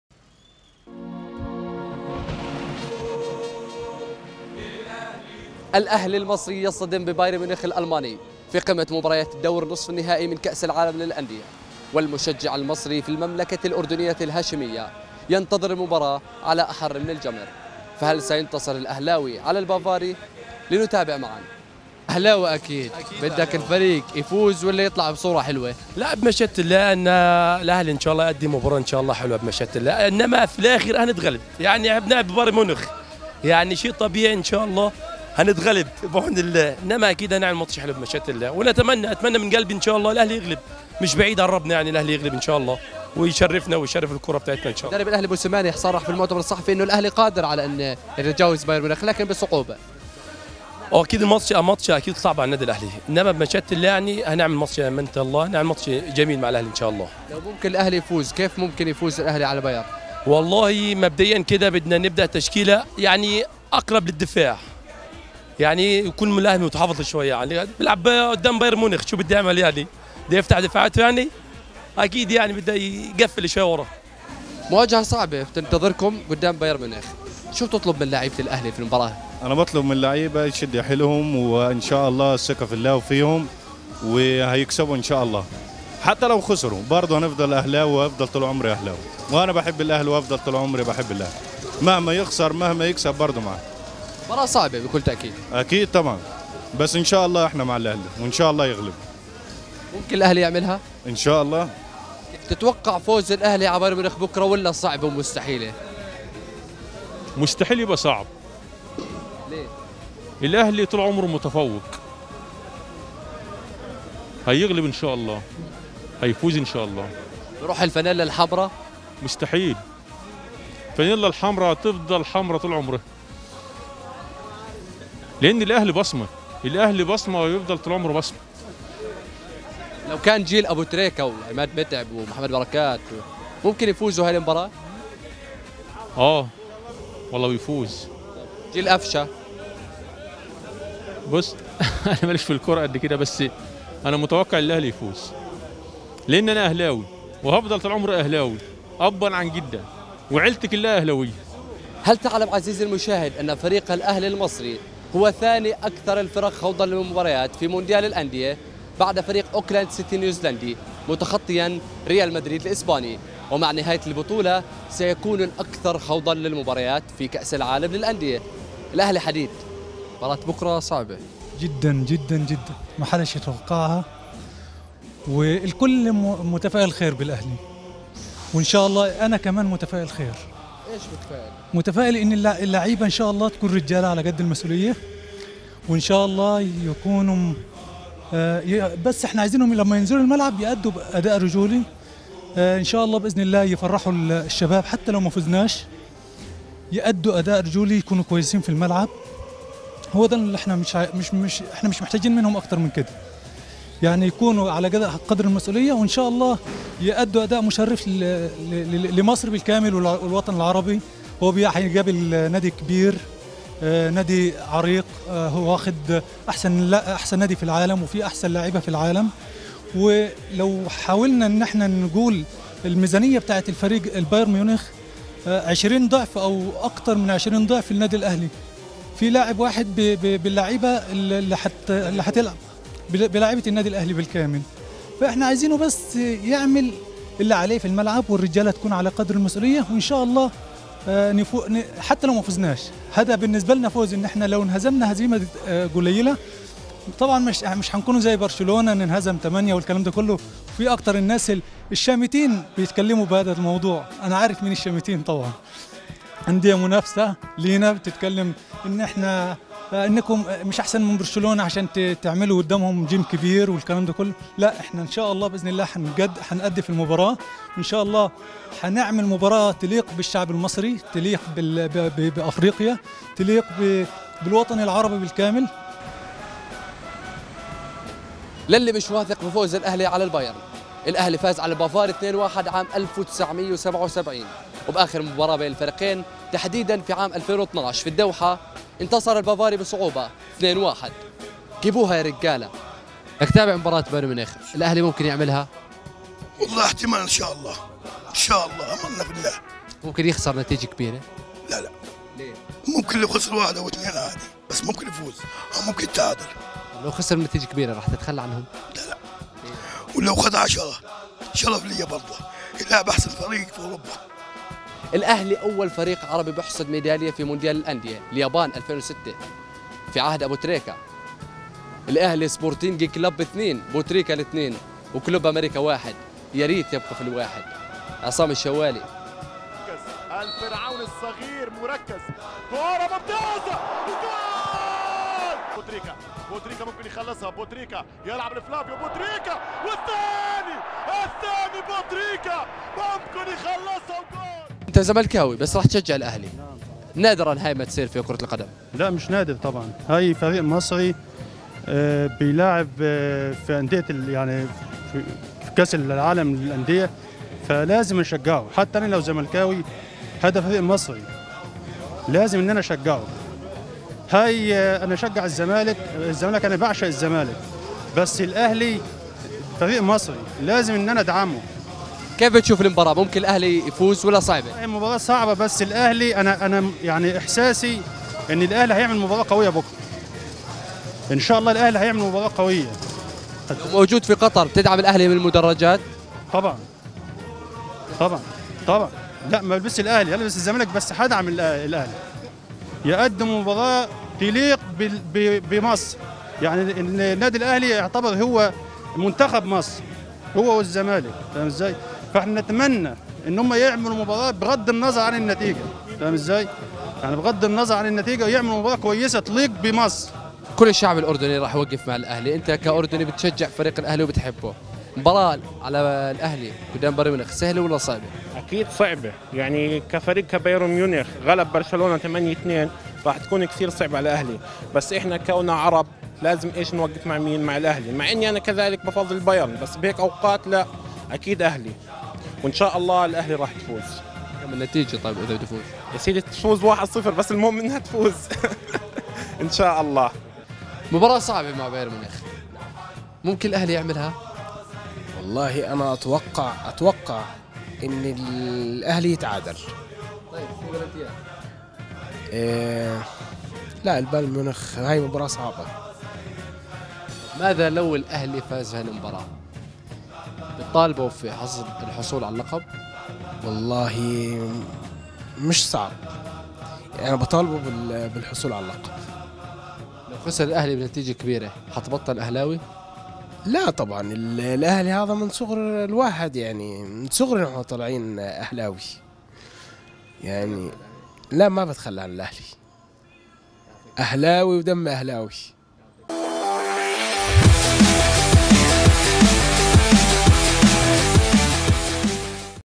آراء جماهير الأهلي في الأردن لمباراة القمة أمام بايرن ميونيخ
الأهلي المصري يواجه بايرن ميونيخ الألماني في الدور نصف النهائي من بطولة كأس العالم للأندية 2021.